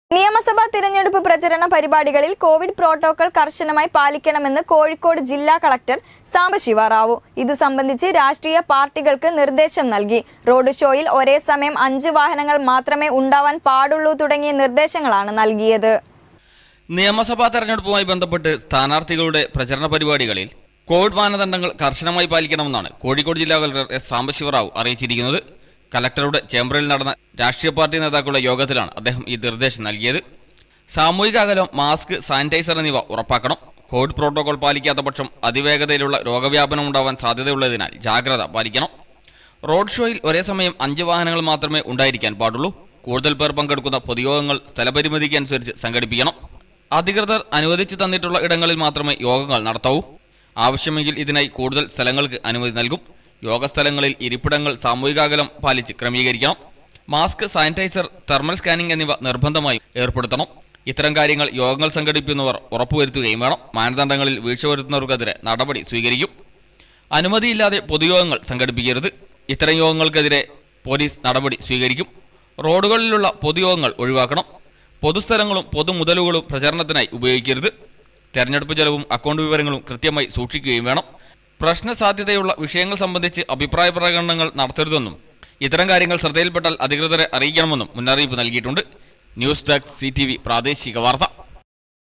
പ്രചരണ പരിപാടികളിൽ കോവിഡ് പ്രോട്ടോക്കോൾ കർശനമായി പാലിക്കണമെന്ന്
നിയമസഭ തെഞ്ഞെടുപ്പ് പ്രചരണ പരിപാടികളിൽ കോവിഡ് പ്രോട്ടോക്കോൾ കർശനമായി പാലിക്കണമെന്ന് കോഴിക്കോട് ജില്ലകലക്ടർ സാംബശിവറാവു. ഇതു സംബധിച്ച് രാഷ്ട്രീയ പാർട്ടികൾക്ക് നിർദേശം നൽകി. റോഡ് ഷോയിൽ ഒരേ സമയം 5 വാഹനങ്ങൾ മാത്രമേ ഉണ്ടാവാൻ പാടുള്ളൂ തുടങ്ങിയ നിർദേശങ്ങളാണ് നൽകിയത്